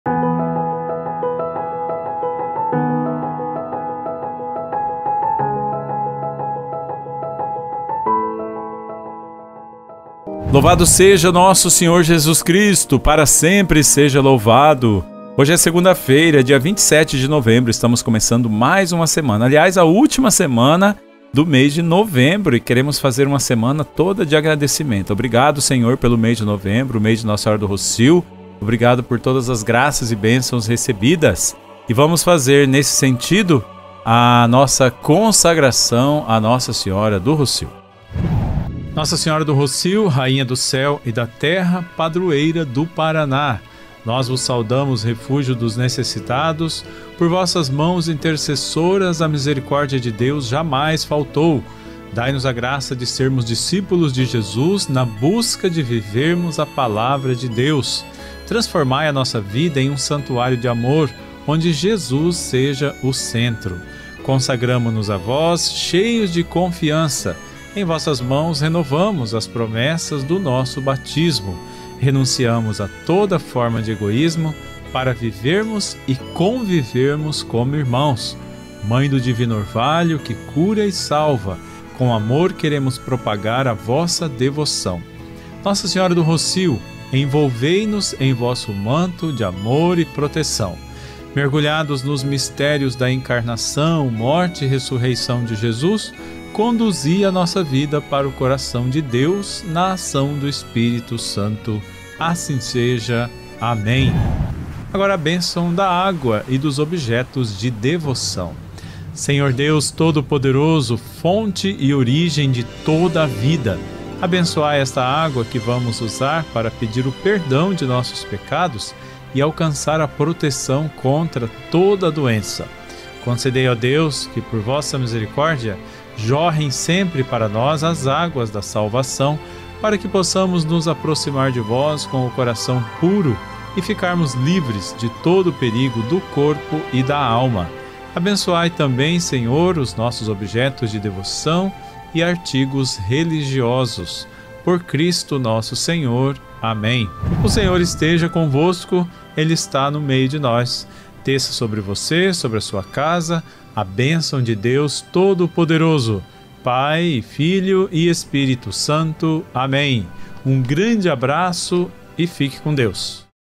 Consagração a Nossa Senhora | 26/11/2023
*Consagração a Nossa Senhora do Rocio*
Nossa Senhora do Rocio, Rainha do Céu e da Terra, padroeira do Paraná. Nós vos saudamos refúgio dos necessitados. Por vossas mãos intercessoras a misericórdia de Deus jamais faltou.
Dai-nos a graça de sermos discípulos de Jesus, na busca de vivermos a Palavra de Deus. Transformai a nossa vida em um santuário de amor, onde Jesus seja o centro.
Consagramo-nos a vós, cheios de confiança. Em vossas mãos renovamos as promessas do nosso Batismo; renunciamos a toda forma de egoísmo, para vivermos e convivermos como irmãos. Mãe do Divino Orvalho, que cura e salva, com amor queremos propagar a vossa devoção.
Nossa Senhora do Rocio, envolvei-nos em vosso manto de amor e proteção. Mergulhados nos mistérios da Encarnação, Morte e Ressurreição de Jesus, conduzi a nossa vida para o coração de Deus, na ação do Espírito Santo. Assim seja, amém!

✓Deixe seu pedido de oração nos comentários, vamos rezar por você!